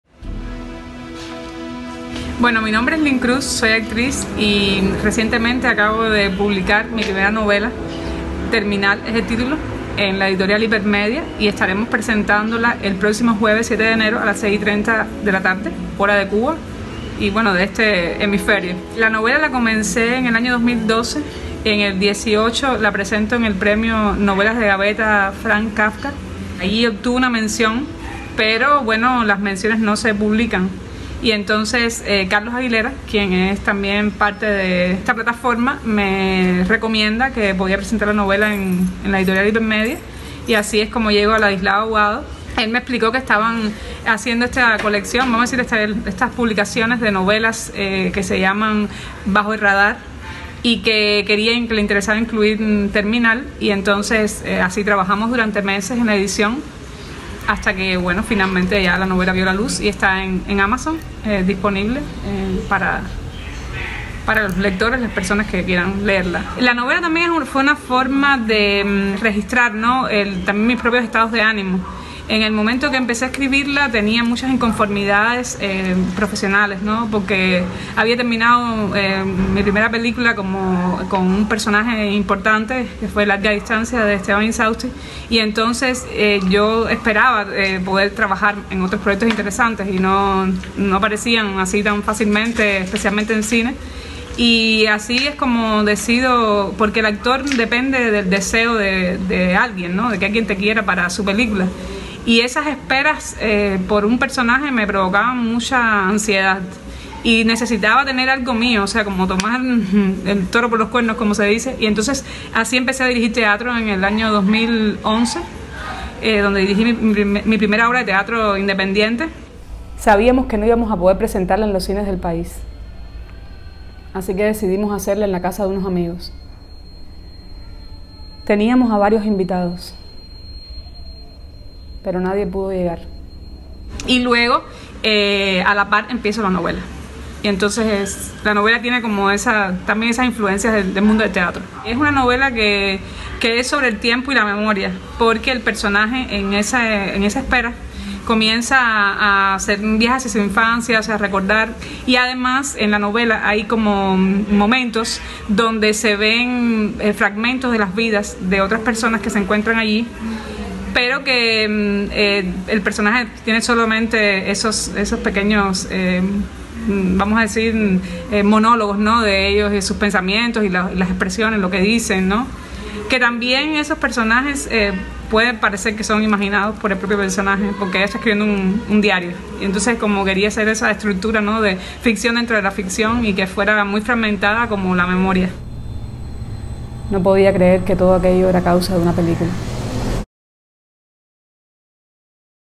“Terminal”, la primera novela de la actriz cubana Lynn Cruz
“Terminal”, la primera novela de la actriz cubana Lynn Cruz, se presenta este jueves en el canal de YouTube y la página de Facebook de la Editorial Hypermedia, responsable de la publicación.